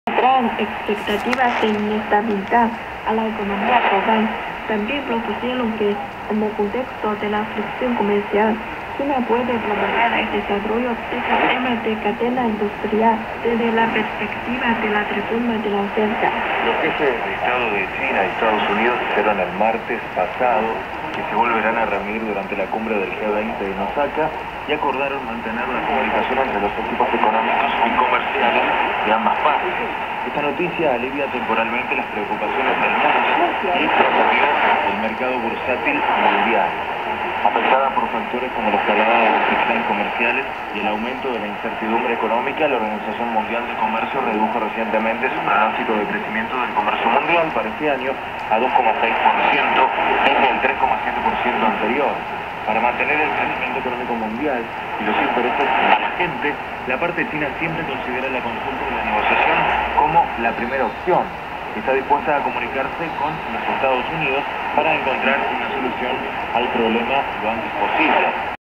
7335 kHz China Radio International - Spanish (Shijiazhuang)
7335  CHINA RADIO INTERNATIONAL
Spanish
PWR 500 AZ 315
22:13 UTC
June 21 2019
TX Shijiazhuang
RX Belo Horizonte
Receiver: Sony7600gr
Antenna: EF-SWL (End-fed)
PY4004SWL